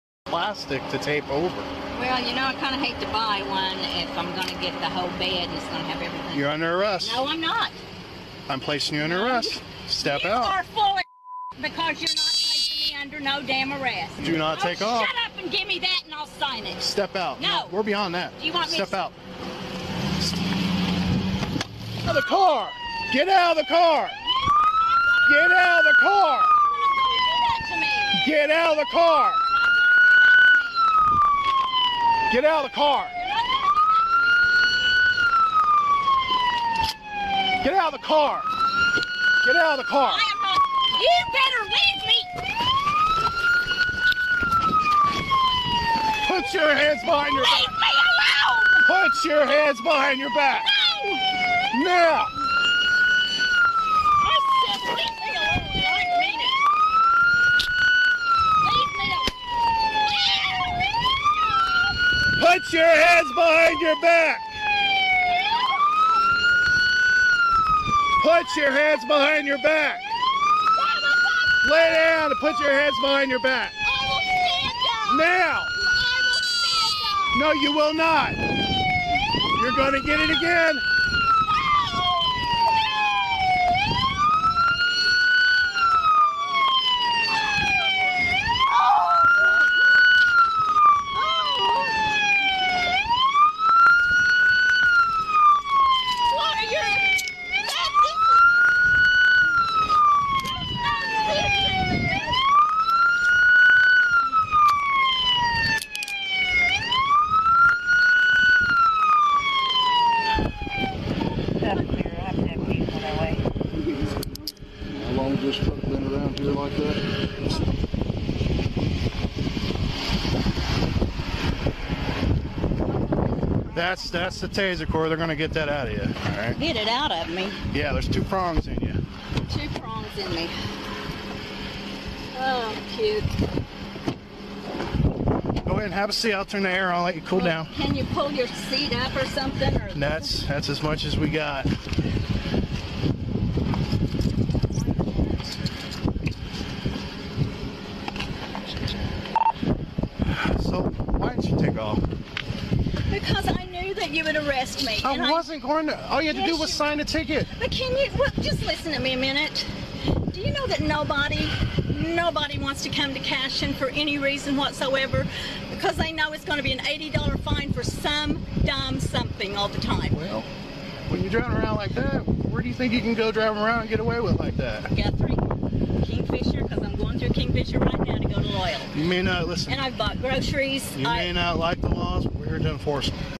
65-year-old Debra resisting arrest and assaulting a police officer
65-year-old Debra was pulled over for a broken tail light in a small city of Oklahoma. She refused to sign the $80 ticket, tried to drive away, and is now facing charges of resisting arrest and assaulting a police officer. 😳😳😳